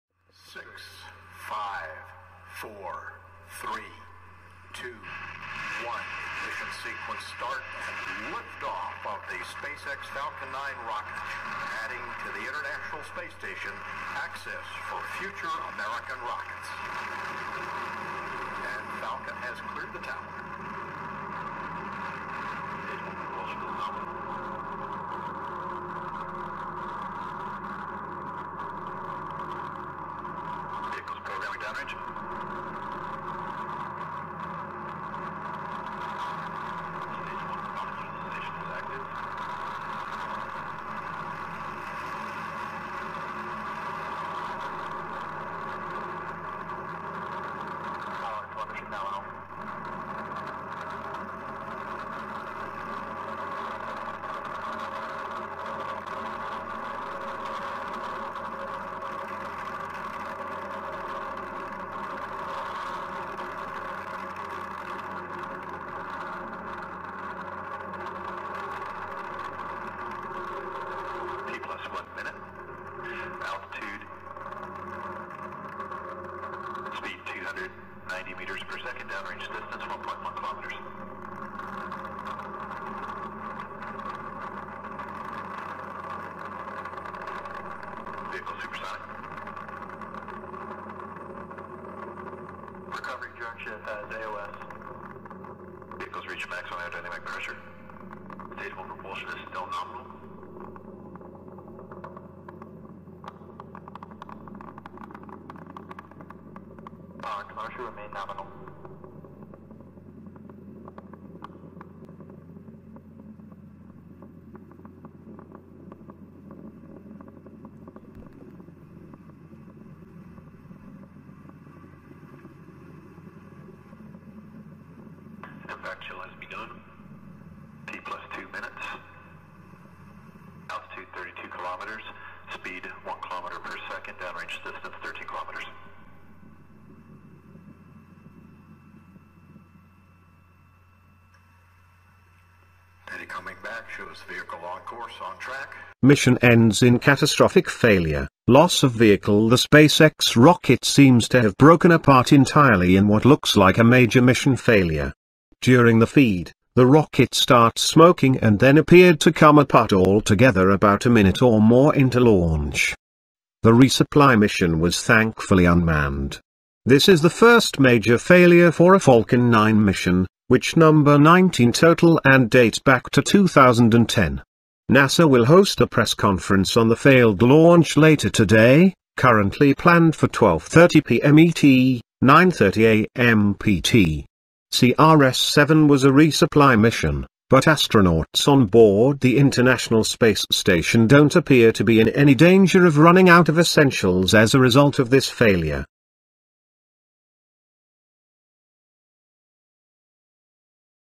SpaceX CRS-7 Mission Ends In Catastrophic Failure 🚀
This is the first major failure for a Falcon 9 mission, which number 19 total and date back to 2010.
NASA will host a press conference on the failed launch later today, currently planned for 12:30 PM ET (9:30 AM PT).
CRS-7 was a resupply mission, but astronauts on board the International Space Station don’t appear to be in any danger of running out of essentials as a result of this failure.